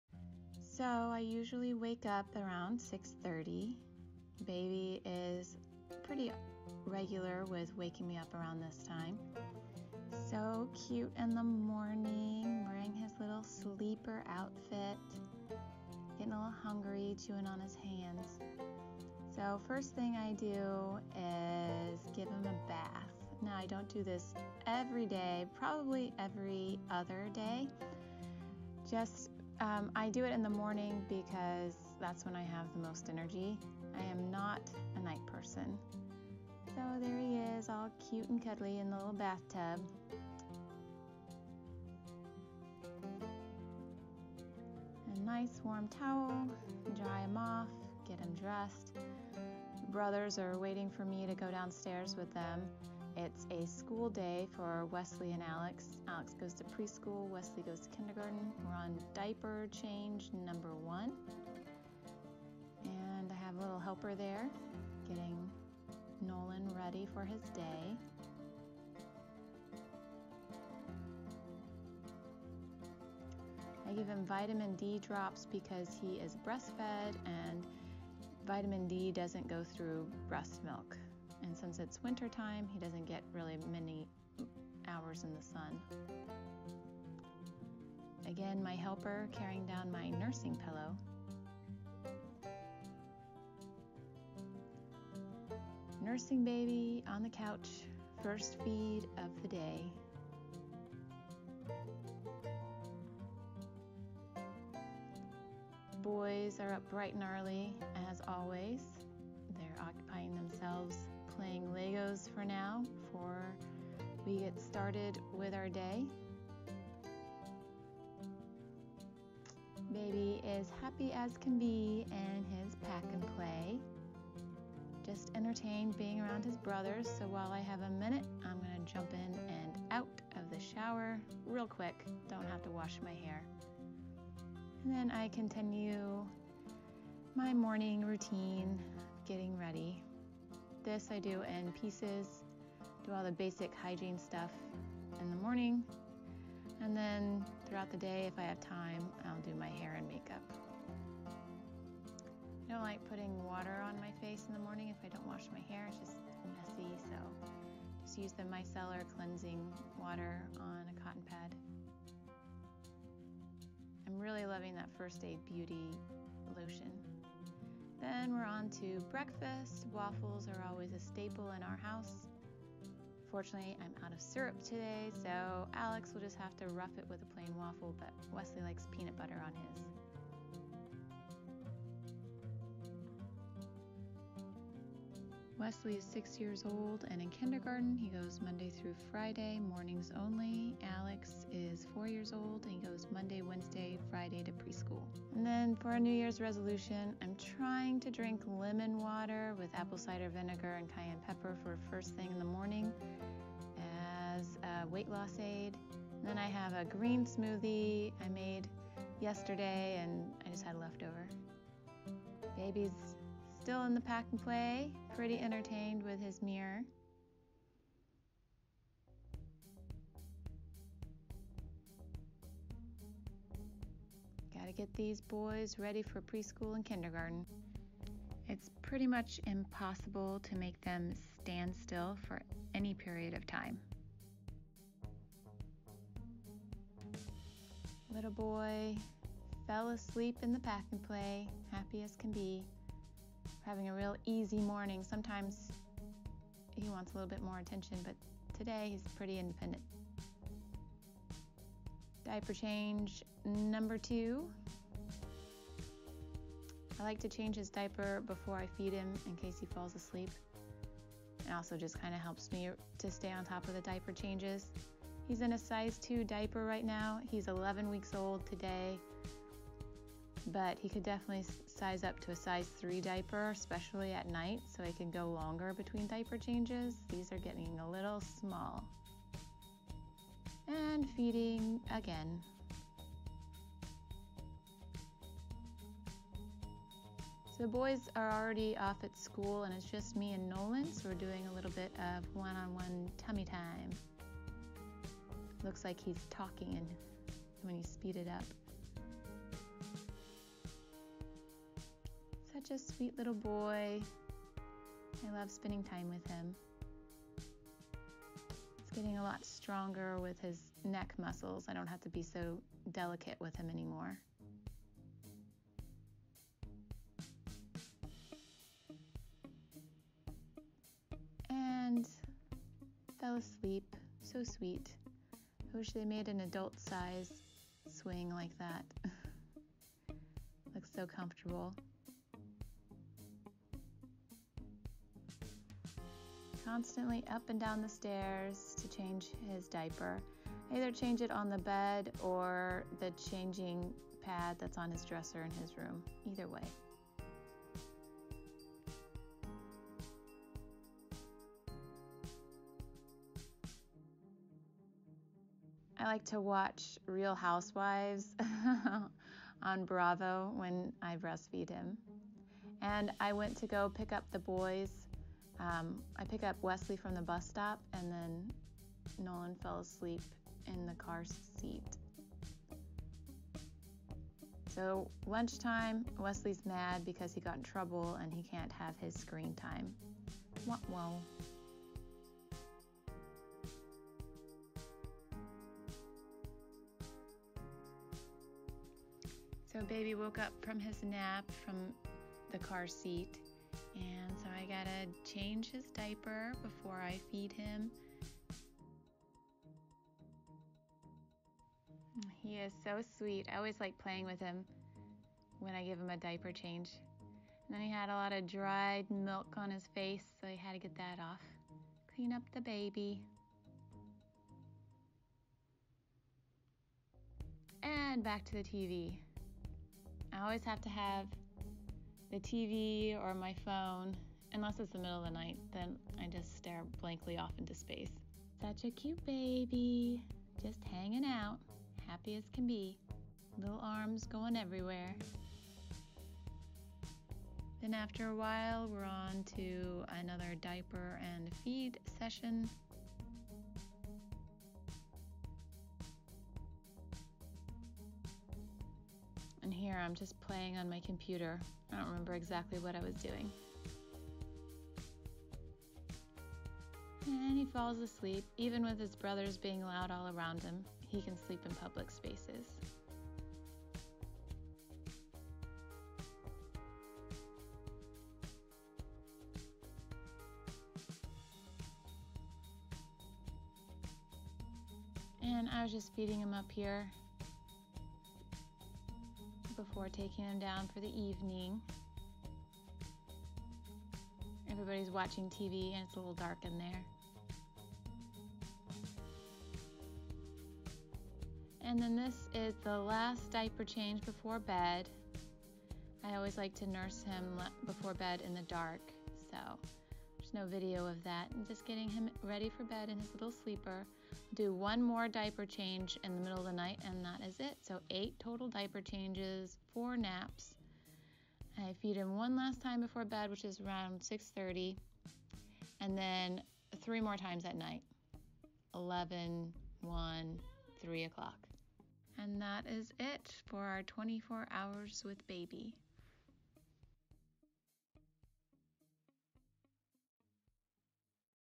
24 HOURS WITH 2 MONTH OLD BABY
Hi! I'm Christina, stay at home mom to Wesley (6), Alex (4) and Nolan (2 months). This is a typical school day routine at our house. I left out dinner and some other activities we do, but I tried to capture what a day with a breastfed baby looks like. In total, there were 7 diaper changes, 9 feedings and 4 daytime naps.

XO
-Christina